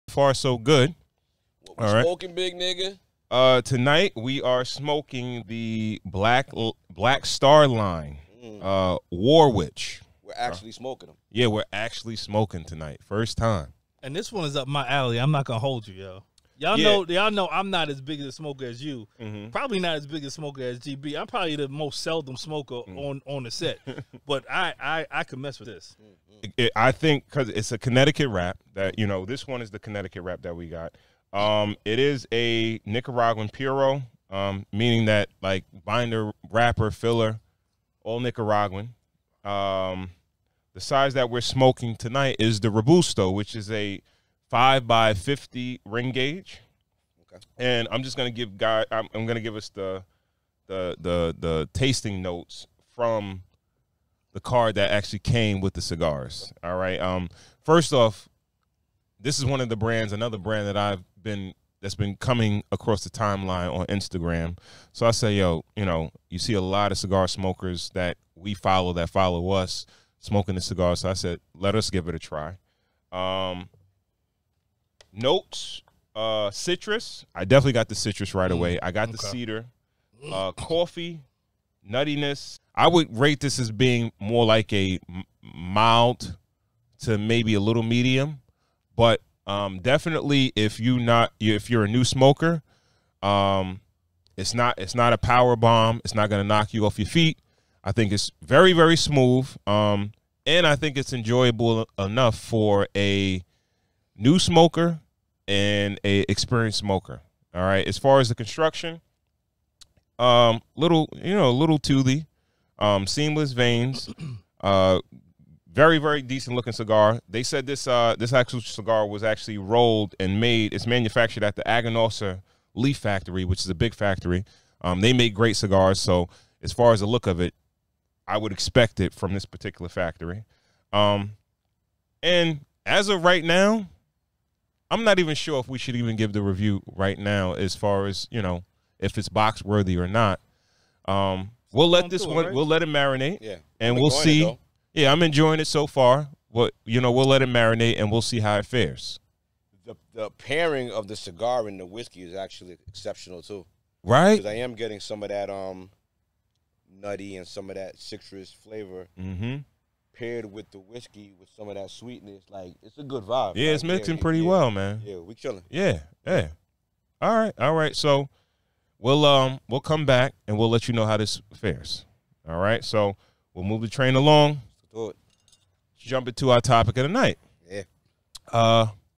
S2E15 Black Star Line War Witch Review
Cigar of the night: Black Star Line War Witch
size: Robusto 5x50
origin: Nicaragua
binder: Nicaragua
wrapper: Nicaragua
filler: Nicaragua